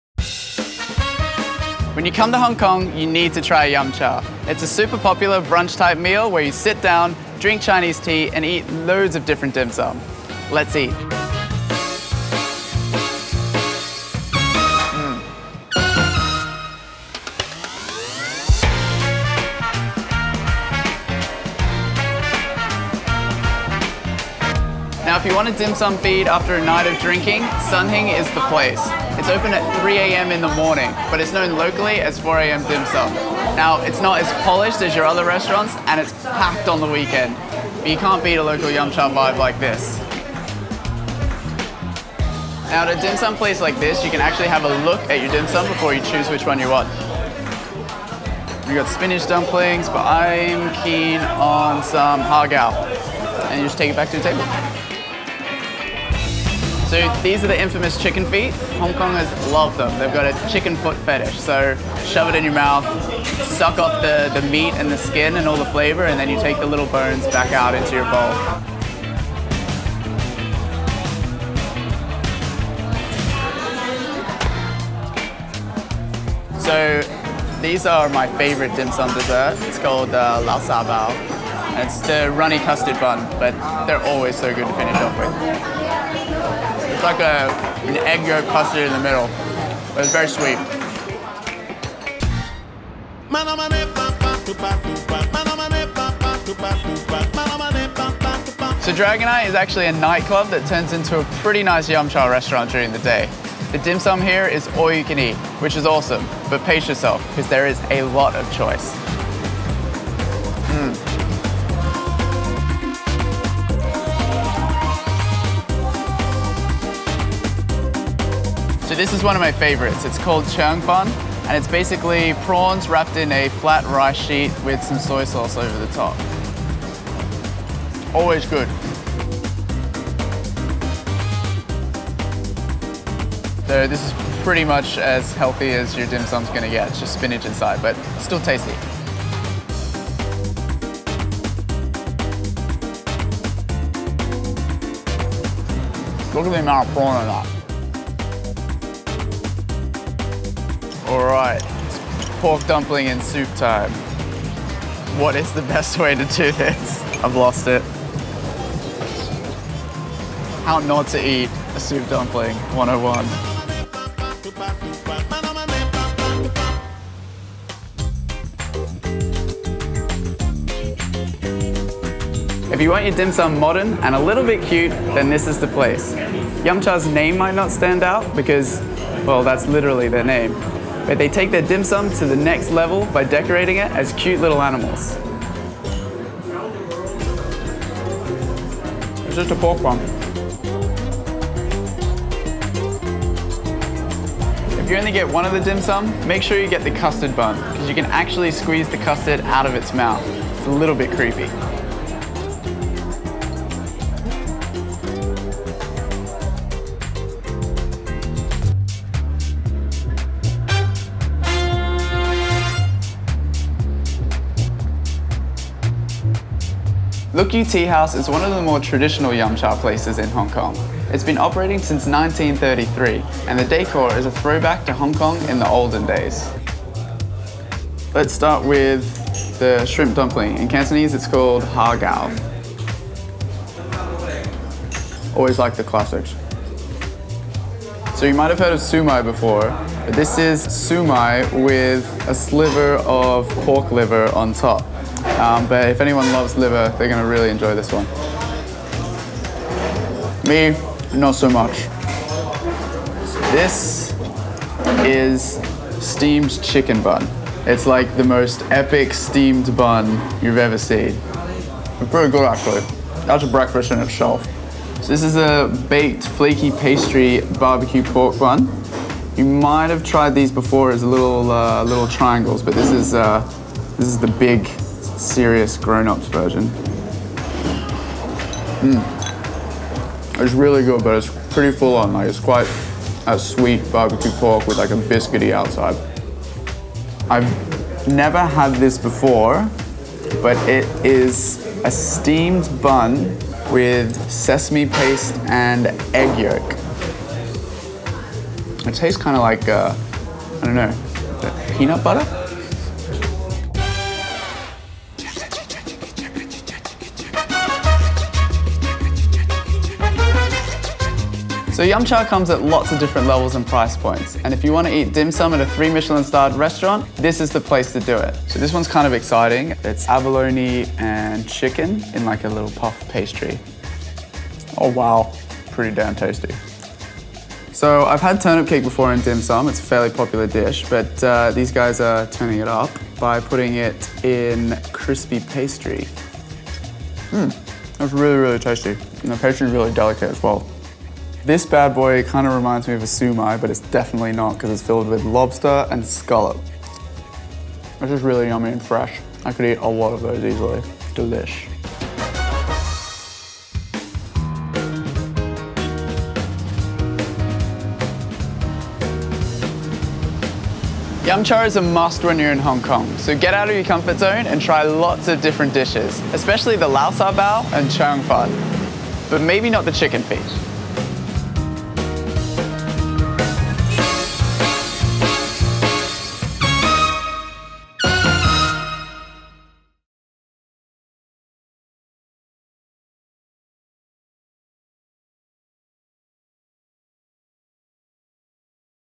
Trying the best places for Yum Cha in Hong Kong!
Having Dim Sum in Hong Kong is a must. Yum Cha is one of Hong Kong’s best meals and there are a lot of amazing places to enjoy it. In this episode of Hawker Style we visit 5 yum cha restaurants to try some of the most popular and unique dishes. Some of the dishes we taste are a massive soup dumpling at Dragon-I, crazy liver siu mai at Luk Yu, Michelin star turnip cake at Lung King Heen, chicken feet at Sun Hing and super cute barbeque pork buns at Yum Cha in Central. Join us as we taste and talk you through some of the must-try yum cha dishes and restaurants in Hong Kong. It’s all in our latest episode of Hawker Style, our original videos series exploring amazing food from across the globe.

ANYDOKO is a digital video channel and magazine dedicated to the visual experience of travel.

FOLLOW ANYDOKO

CREDITS

Director & Writer: Vikash Autar
Editor: James Gilligan
Presenter: Dean Vowles